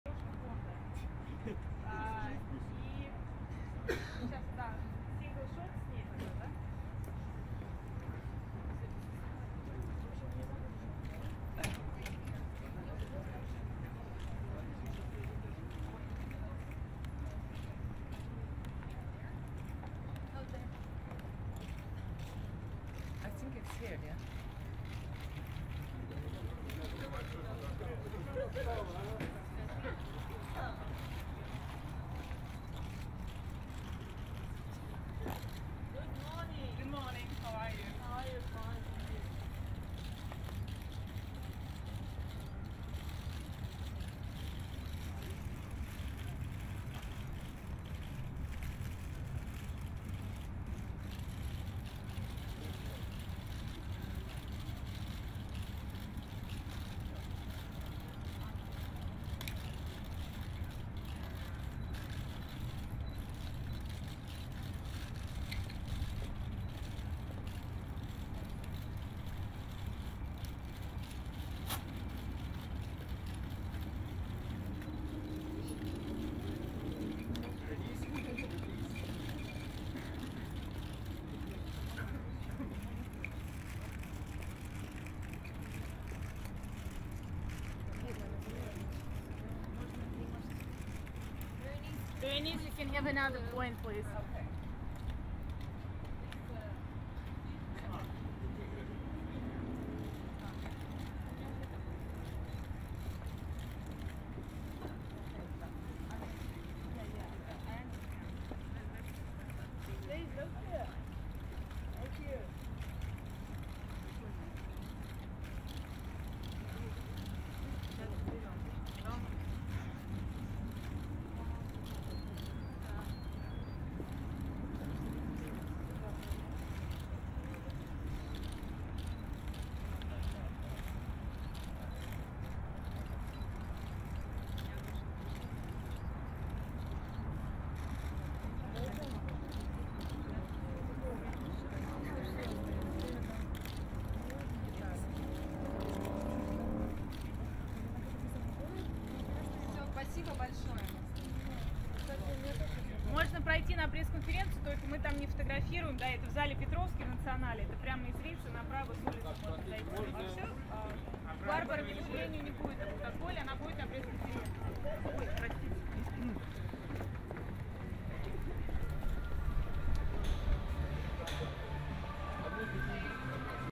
Беренис Марло в Москве. Фотоколл
Новая девушка Бонда - актриса Беренис Марло прилетела в Москву в компании с продюсером Барбарой Брокколи представить фильм "007: координаты "Скайфолл" (Skyfall).